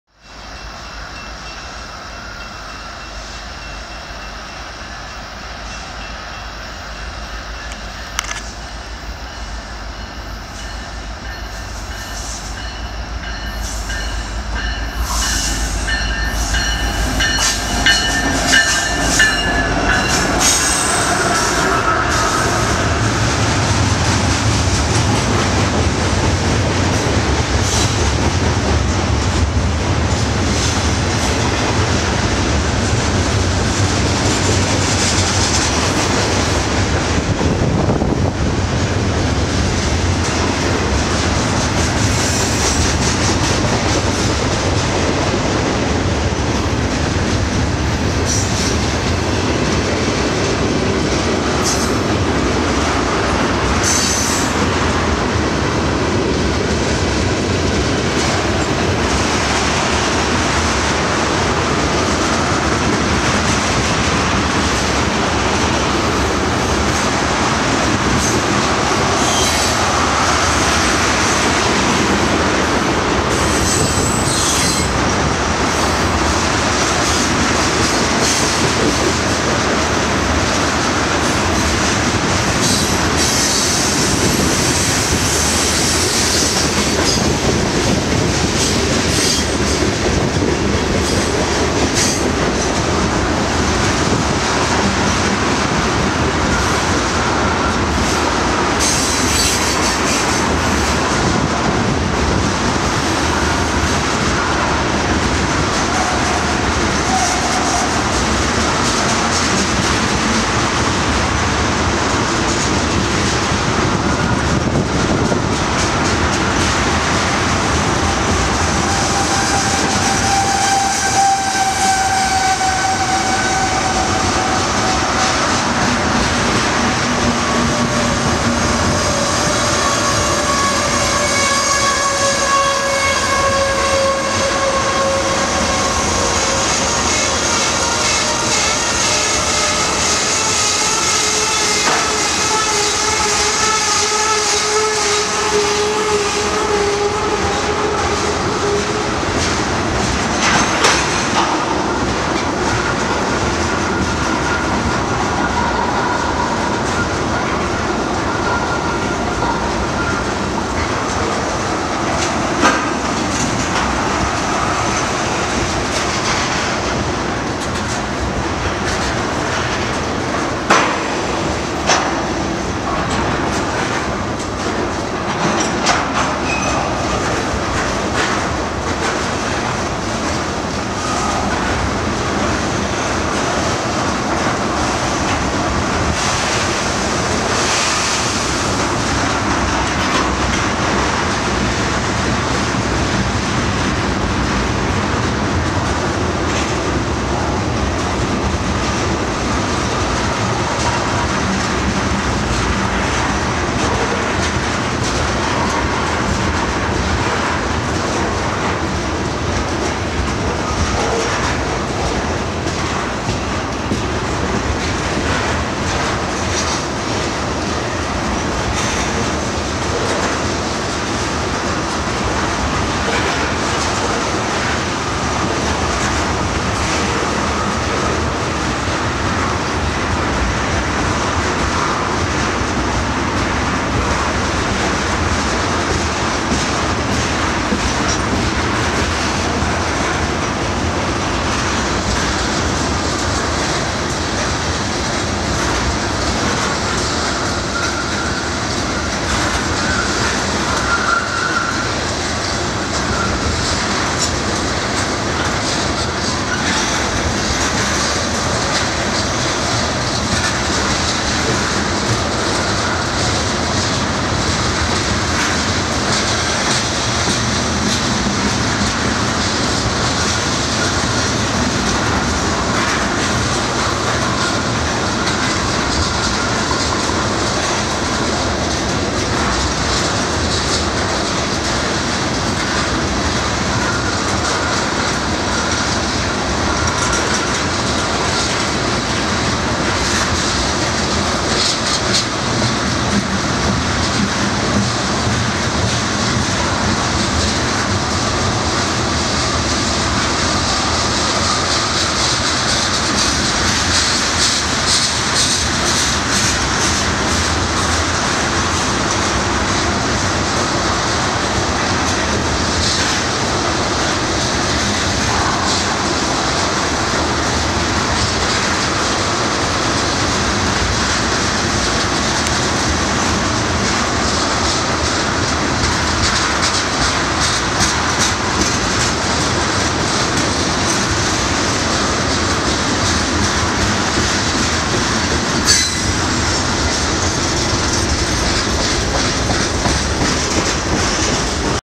NS 1145 East - Tinley Park, IL
NS SD70ACe 1145 leads IAIS train CBBI east through Tinley Park.